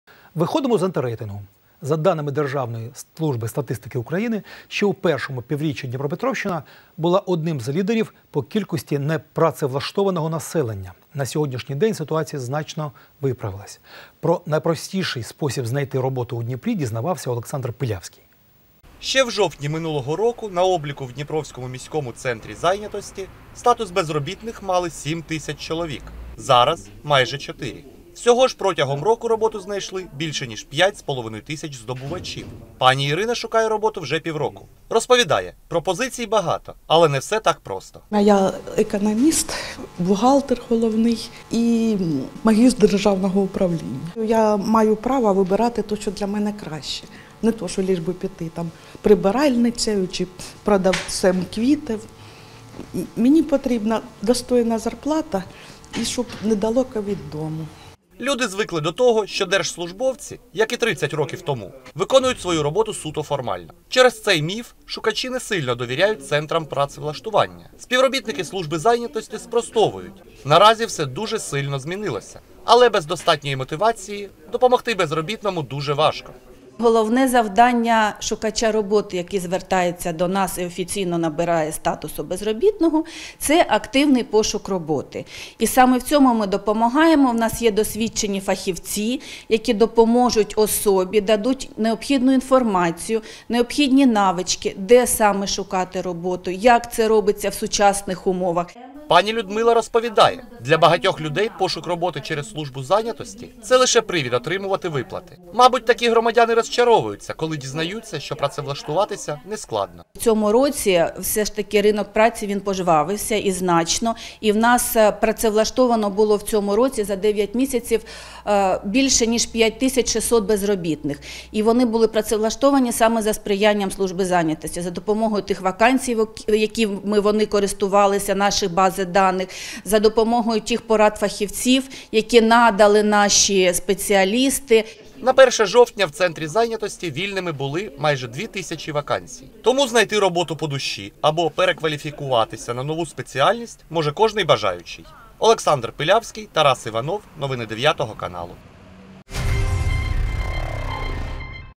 Наскільки важко знайти в Дніпрі роботу, і хто може в цьому допомогти
За даними Державної служби статистики України, ще у першому півріччі Дніпропетровщина була одним з лідерів по кількості не працевлаштованого населення. На сьогоднішній день ситуація значно виправилась. Про найпростіший спосіб знайти роботу у Дніпрі у сюжеті